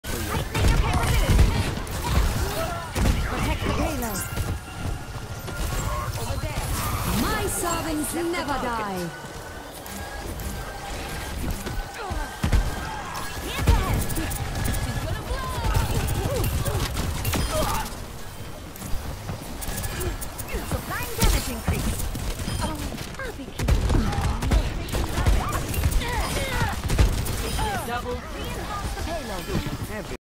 I Give The Gift Of Mercy To My Team, And Theirs.
Get offensive while keeping everyone alive with targeted healing and quick dashes, Valk is Broken.